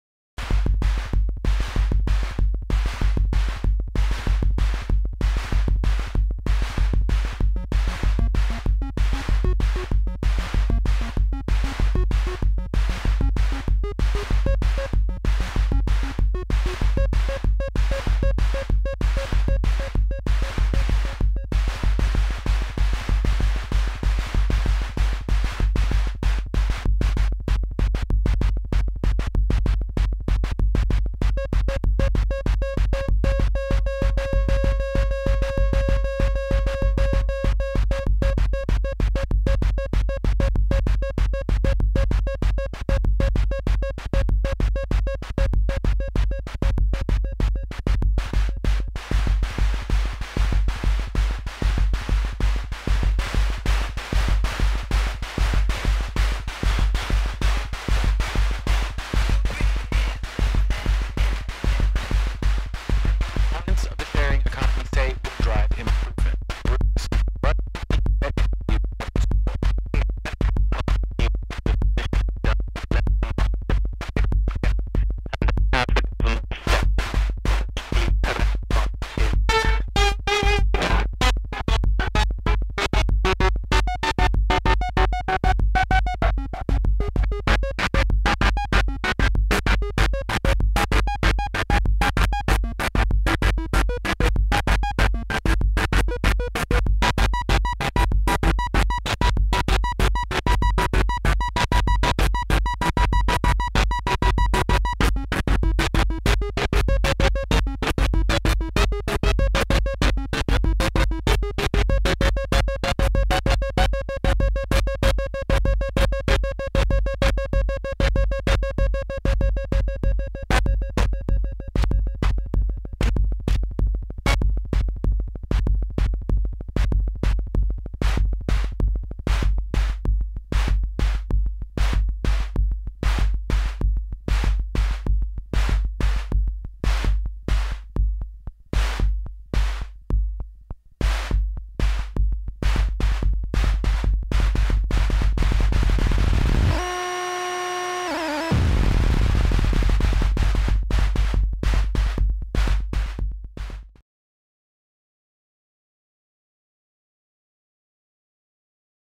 Radio Ace - Mystery Circuits AMFM Drum Machine
Radio Ace is a programmable drum machine built by Mike Walters that uses an AM/FM radio for the source of the snare drum noise. There's an analog bass drum, and it gates an input in time with the beat. Snare (radio), and Input have individual adjustable sustain. Tempo control can go up to audio frequencies. Press the SNOOZE bar to turn off/on the rhythms! Built for Mark Lewis, 2015